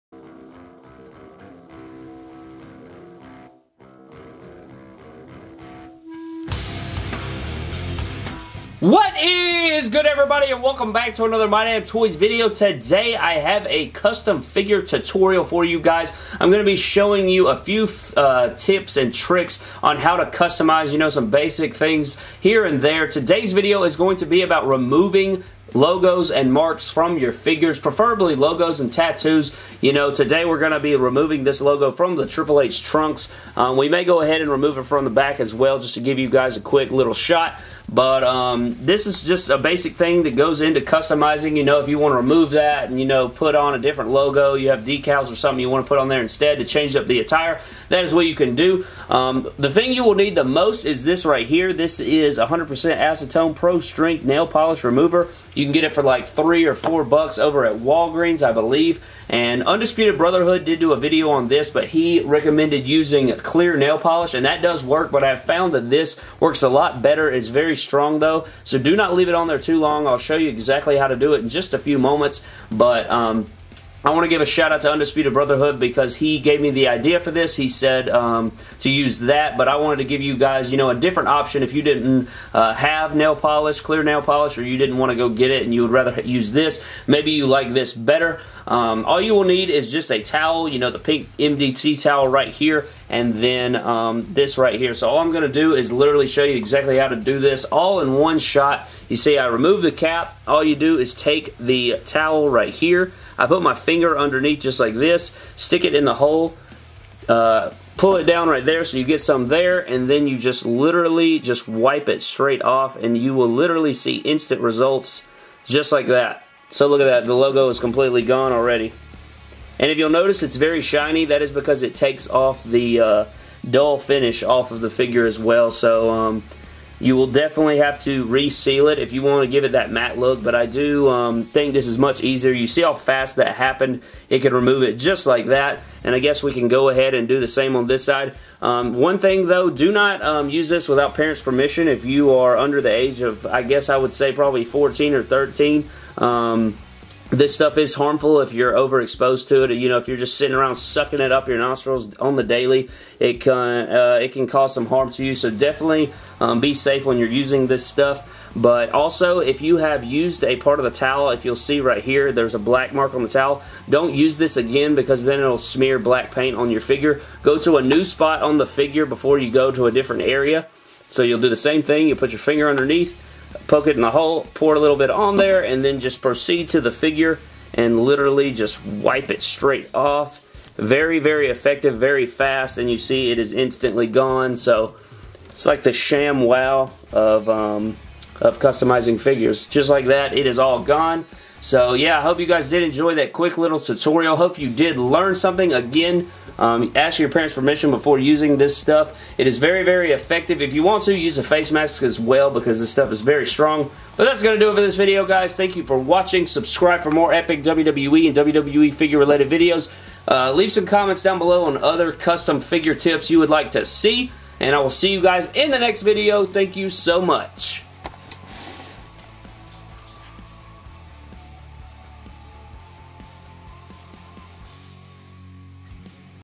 HOW TO REMOVE LOGOS & TATTOOS FROM YOUR WWE FIGURES!
GET ALL YOUR EPIC WWE FIGURES AT WALKOUTWEAR.COM